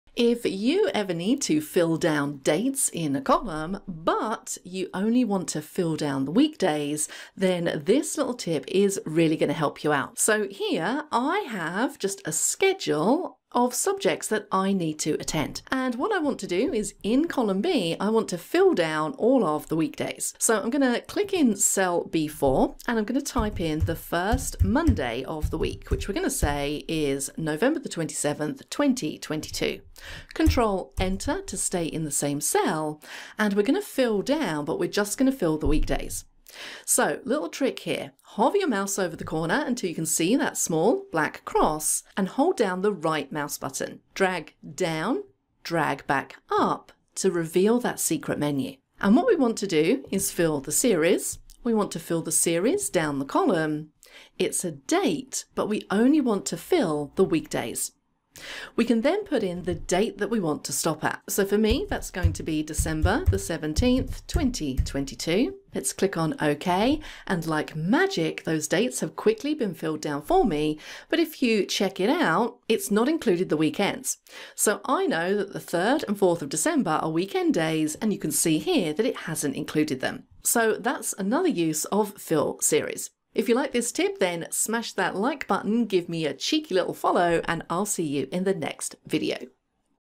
⬇️Fill Down a Series of Dates in Excel Excluding Weekends – Easy Excel Hack☝️
Use the Fill Series feature in Excel to quickly fill down consecutive dates BUT exclude the weekends.

In this example, we are using a schedule of subjects that a student needs to attend each day. Students only attend classes on weekdays. We need to complete the dates in the schedule in the most efficient way but exclude the weekends.